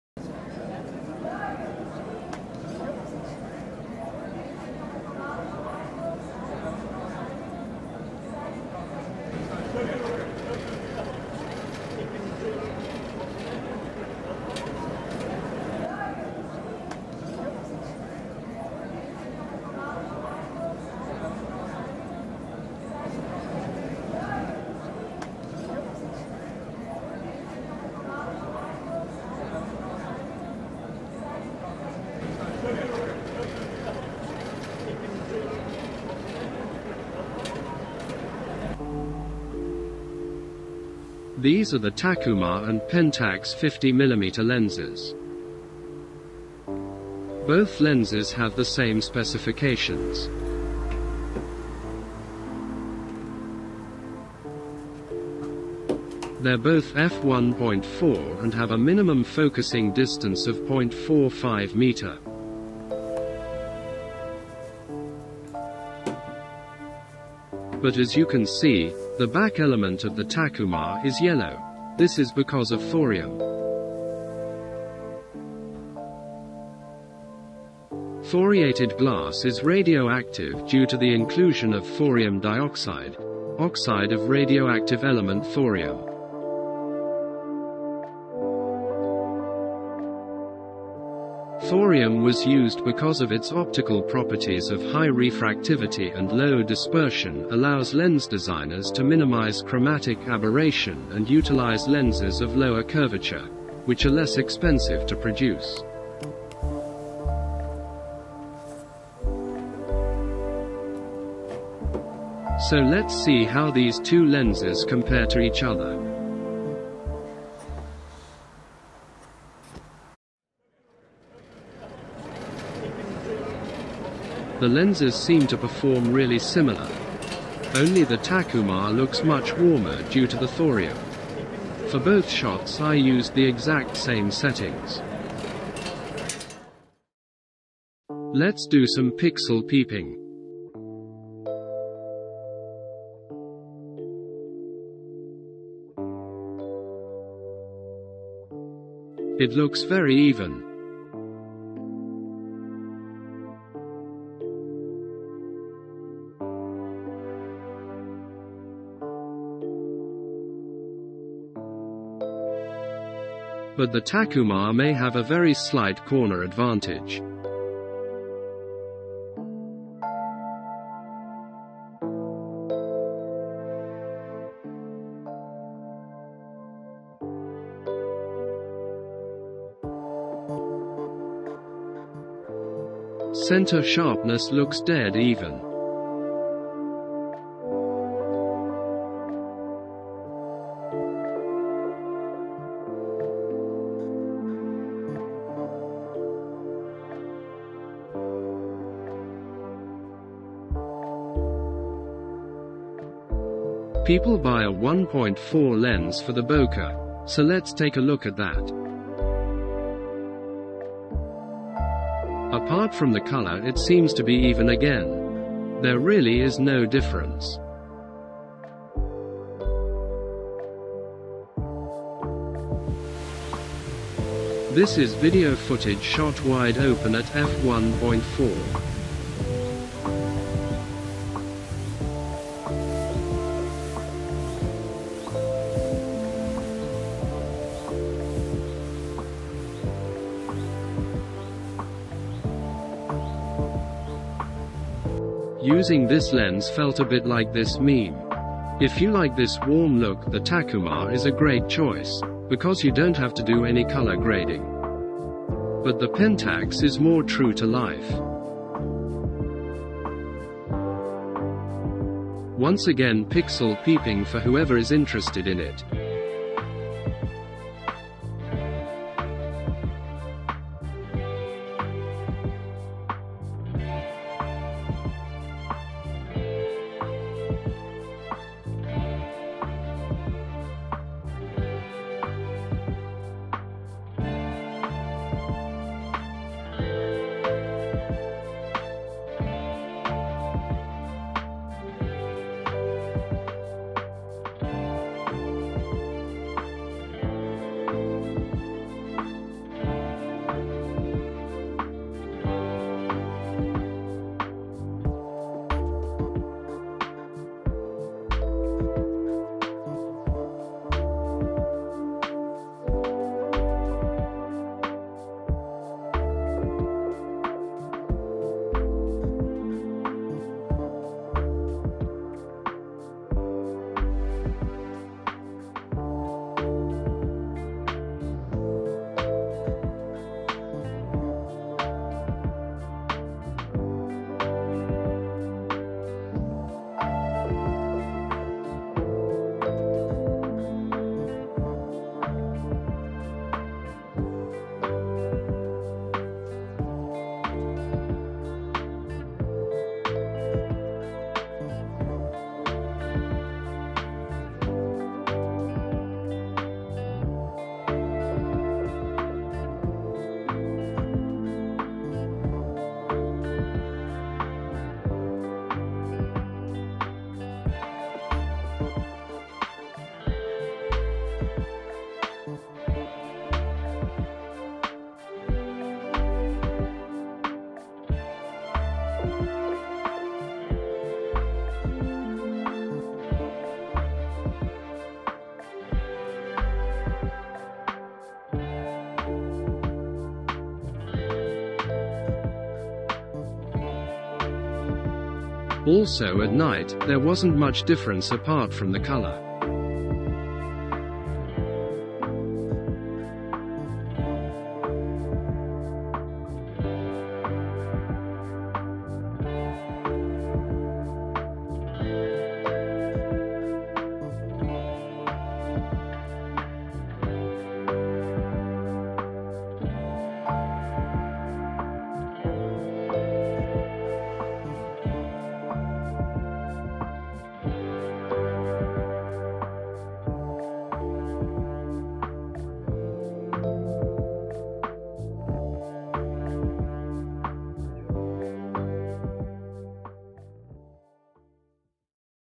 Takumar vs Pentax-M, thorium vs non-thorium. Which 50mm f1.4 lens is better? (Radioactive lens)
A quick comparison between two fast 50mm lenses from Pentax. The SMC Takumar (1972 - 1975) contains thorium and thus is radioactive. Does this make the lens better than the later Pentax-M version? I did a quick test to see which lens performed better.

Spoiler: performance is the same, only because of thorium the Takumar is much "warmer"